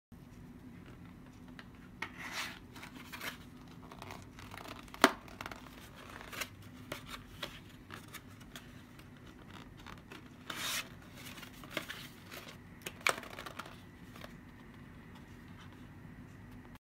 pillowbox folding SoOPAK
How to fold a pillow box. Pillowbox styles are a simple and easy packaging solution for your unique product and work within a variety of different industries.